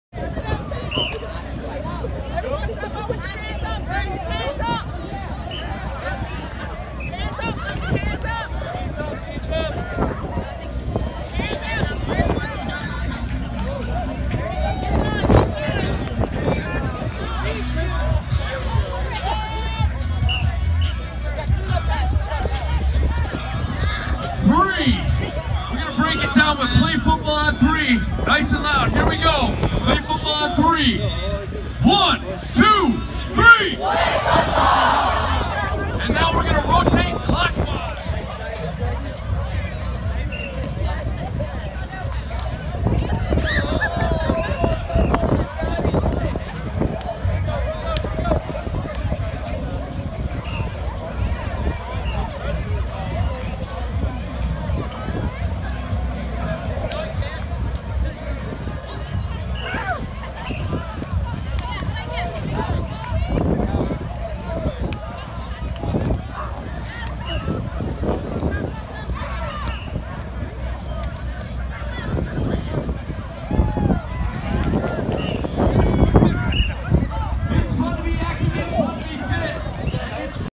Josh Allen works with kids at NFL Play Football event
Former University of Wyoming quarterback Josh Allen, along with several other NFL draft prospects, works with kids during the NFL's Play Football event on Wednesday, April 25, 2018, outside of AT&T Stadium in Arlington, Texas. Video by Robert Gagliardi/WyoSports